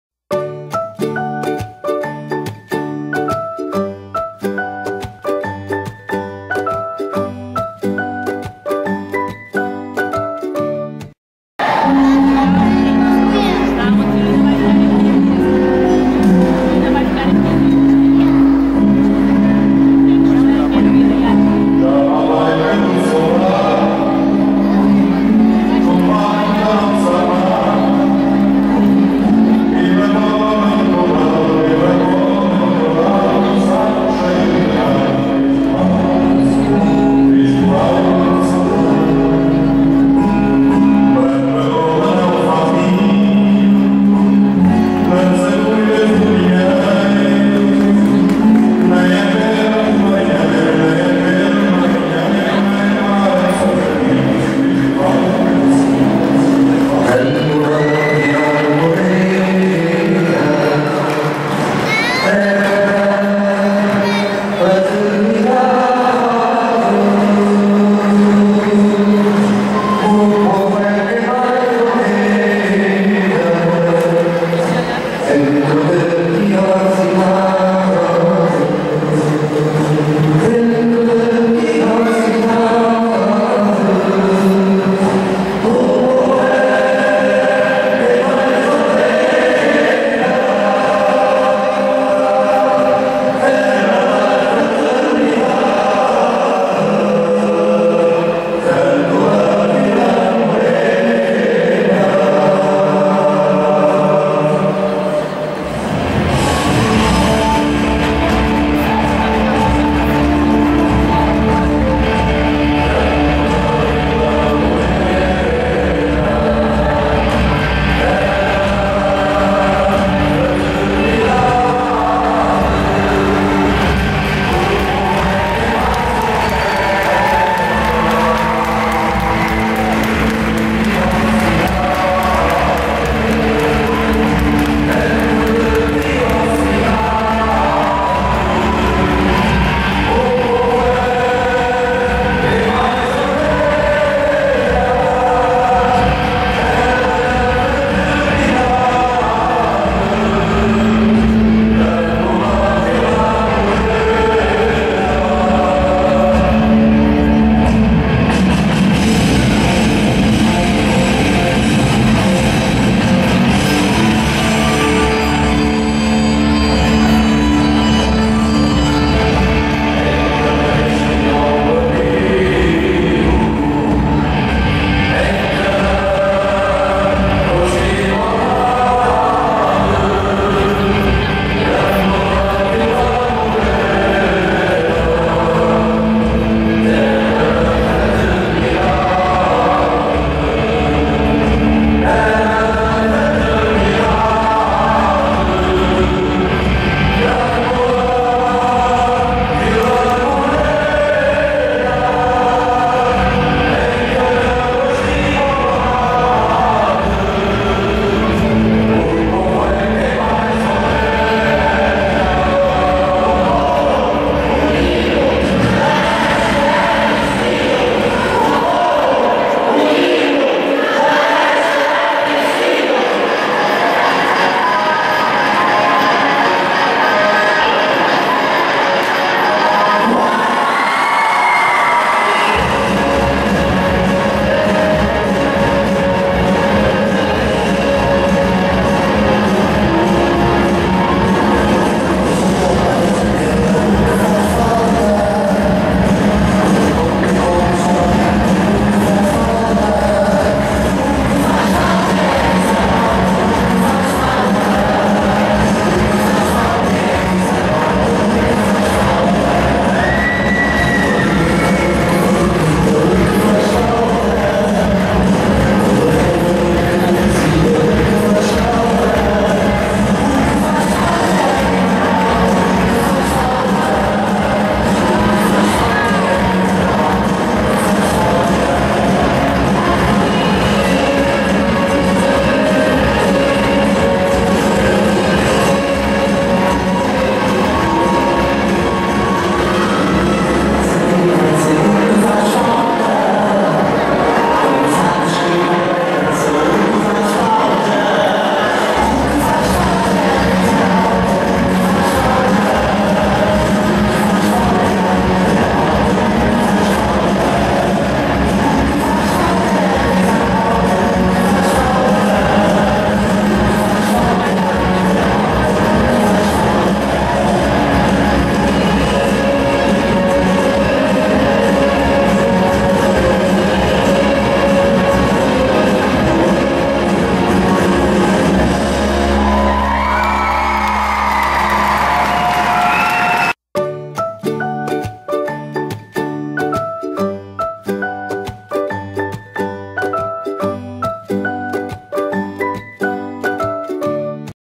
Hóquei Clube da Mealhada - Revolução dos cravos
IV festival de Patinagem Artística do Hóquei Clube da Mealhada
2 de dezembro de 2017